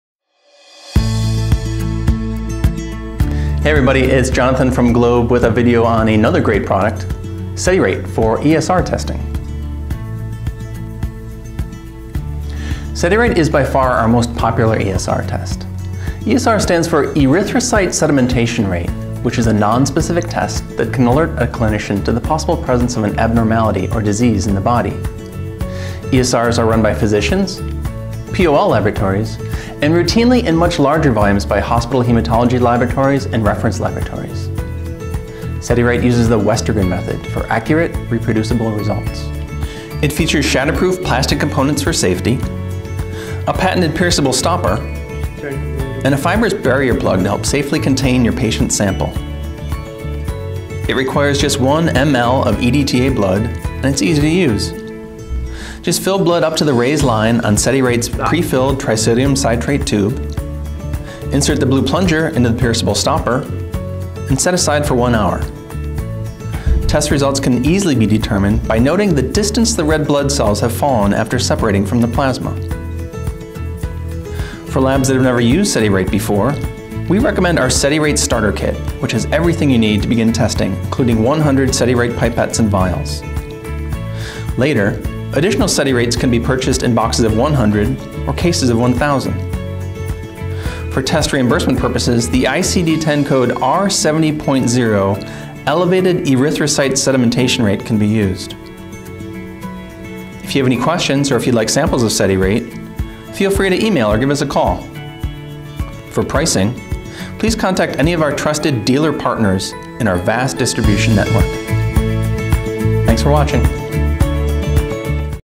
Sedi-Rate ESR System from Globe Scientific
Sedi-Rate offers a simple, safe, economical and highly accurate method for performing Westergren determinations.